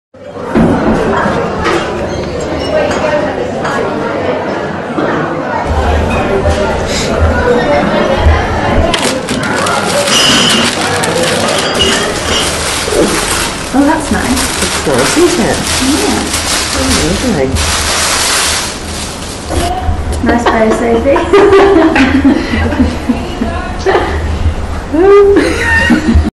Opening an Oliver Harvey Order
This video was created by the Petit Delight Brasserie in Cleethorpes shows exactly what opening an Oliver Harvey order looks like with all of our signature touches; the Product Guide, Sharpie and Air Freshnher